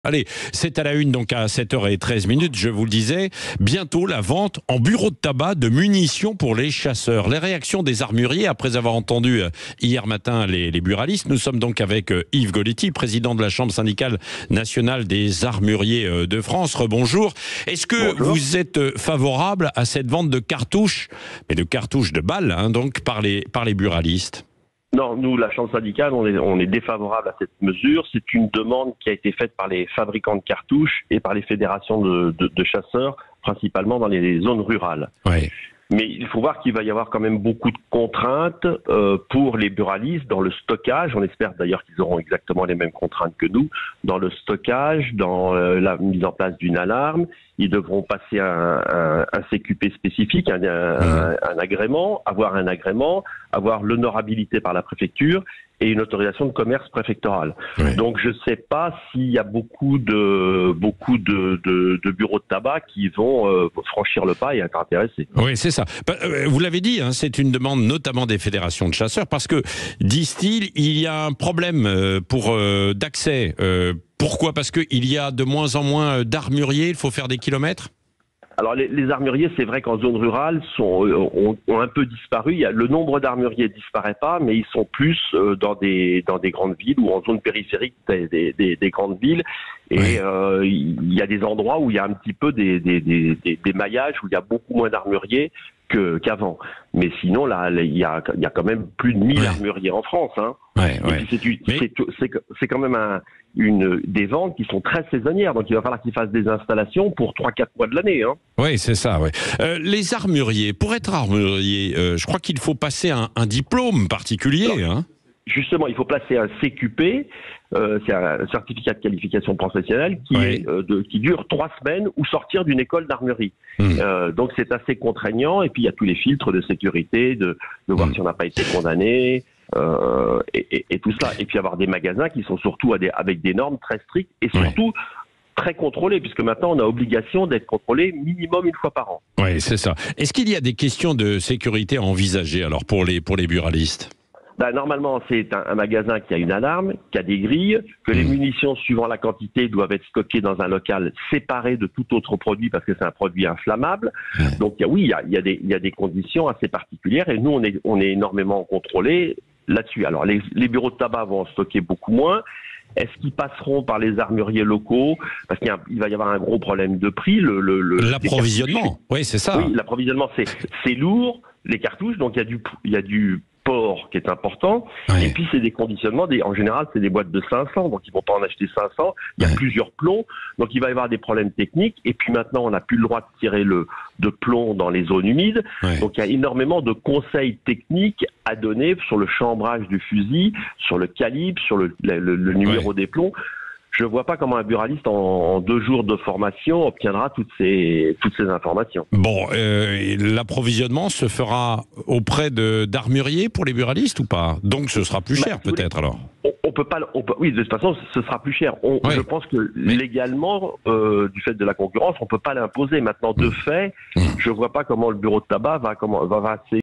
Des munitions bientôt au bureau de tabac : la réaction des armuriers
Avec Yves Gollety, Président de la Chambre syndicale nationale des armuriers de France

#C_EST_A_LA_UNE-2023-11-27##